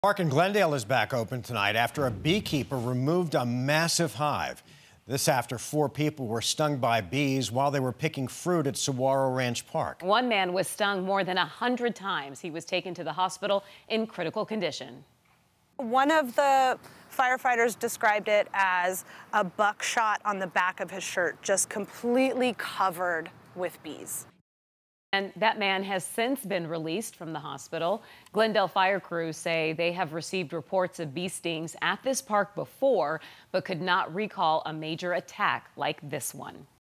Glendale park reopens after massive beehive removed
Saguaro Ranch Park in Glendale has reopened after a beekeeper removed a massive hive. Four people were stung by bees while picking fruit at the park.